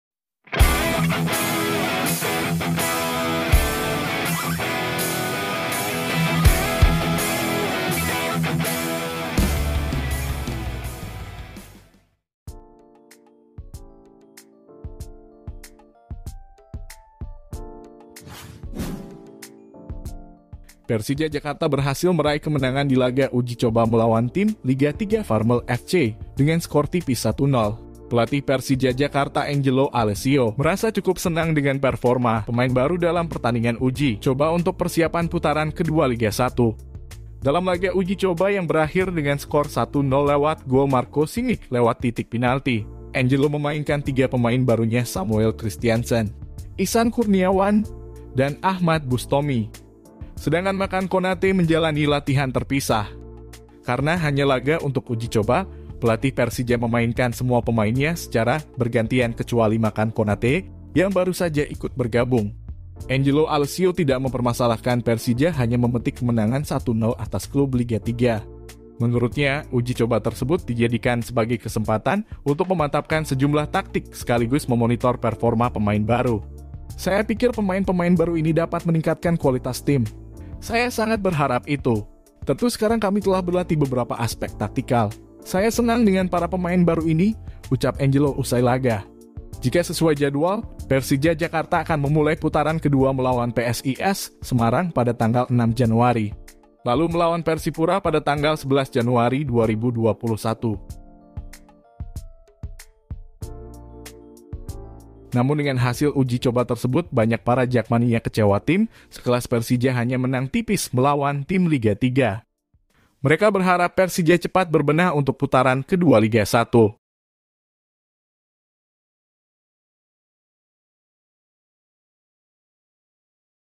Persija Jakarta Cuma Menang Tipis Lawan Tim Liga 3! Ini Kata Angelo Alessio
Persija Jakarta melakukan ujicoba melawan tim liga 3 farmel FC untuk persiapan putaran kedua liga 1